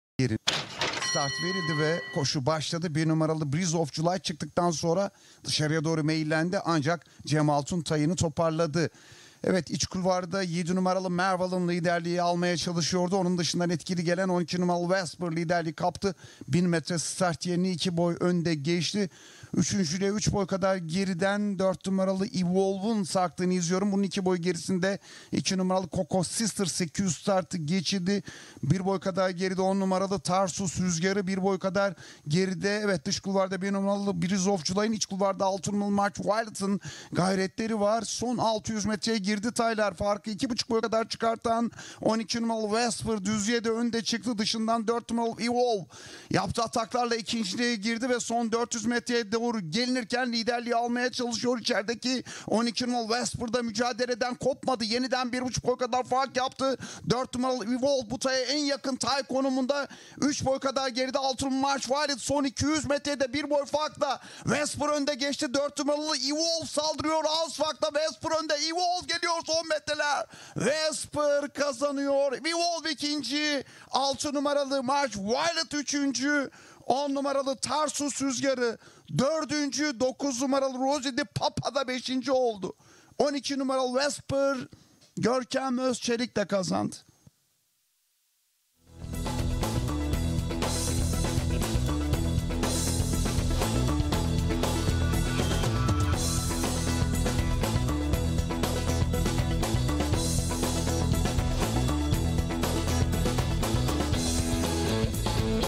🥇Vesper by 𝐑𝐄𝐃 𝐆𝐈𝐀𝐍𝐓
Maiden
🇹🇷 Istanbul
1200mt Kum

🥇Vesper
(𝐑𝐄𝐃 𝐆𝐈𝐀𝐍𝐓 - Queen Nova by Choisir)

Sahip: Oğuz Dağlaroğlu
Antrenör: Aydın Güven Gönültaş
Jokey: Görkem Özçelik
Yetiştirici: Oğuz Dağlaroğlu